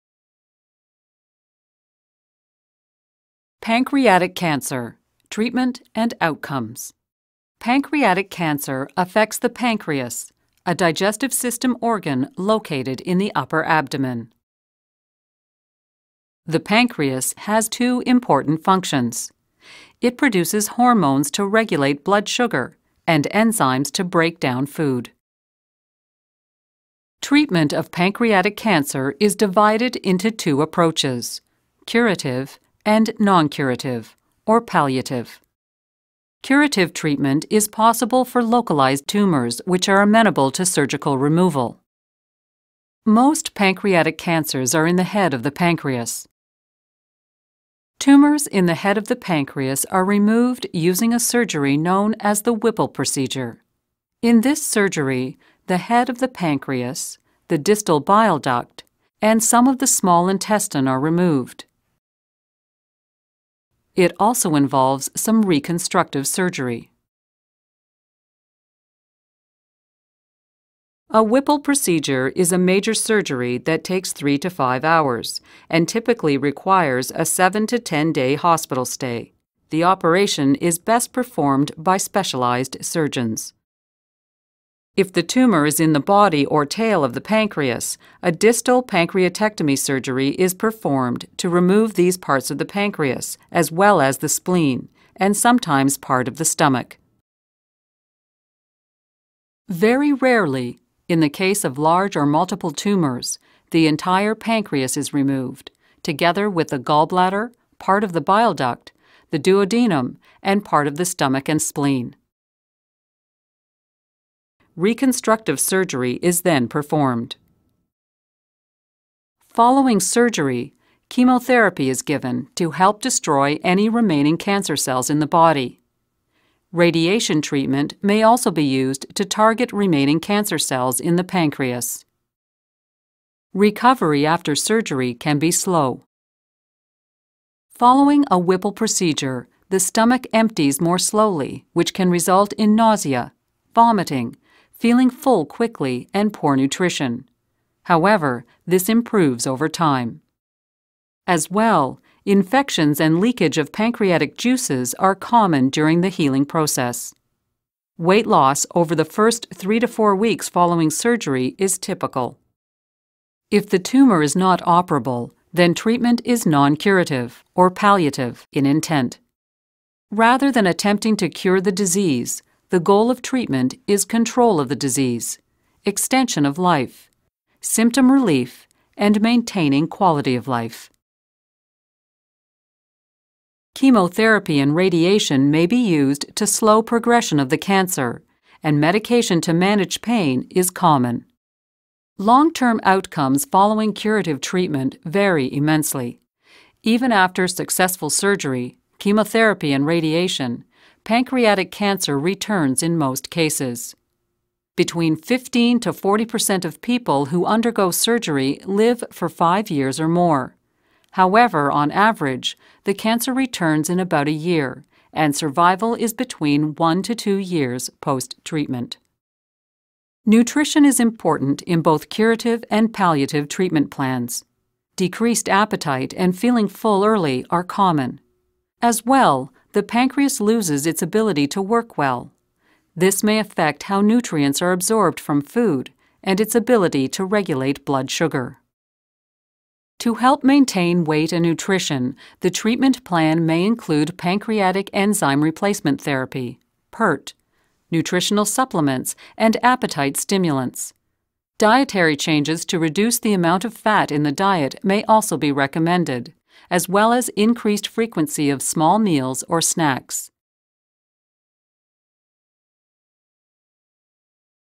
Pancreatic Cancer: Treatment and Outcomes
This animation focuses on two approaches in the treatment of pancreatic cancer: curative and non-curative (palliative), as well as its long-term outcomes.